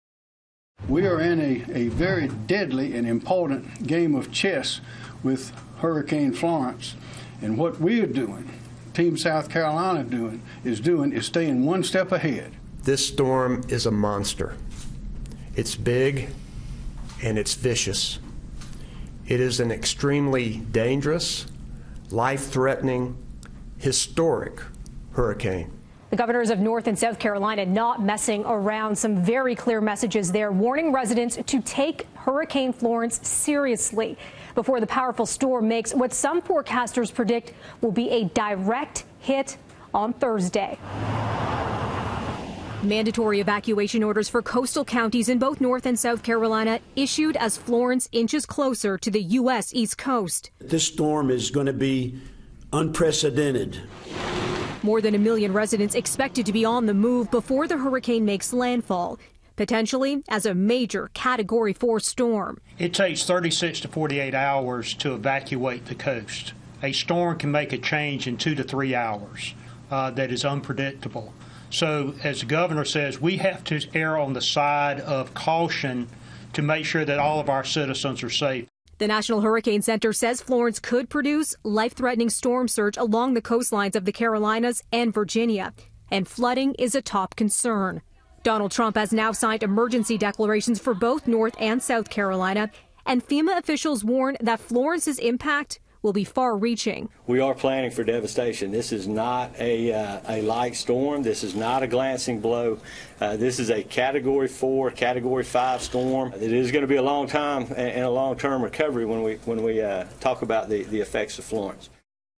'Monster' Hurricane Florence taking aim at U.S. east coast
The U.S. east coast is bracing for Hurricane Florence, and South Carolina’s governor is describing the situation as a “deadly game of chess.” Melissa Duggan with the preparations ahead of what’s being called a “monster” hurricane.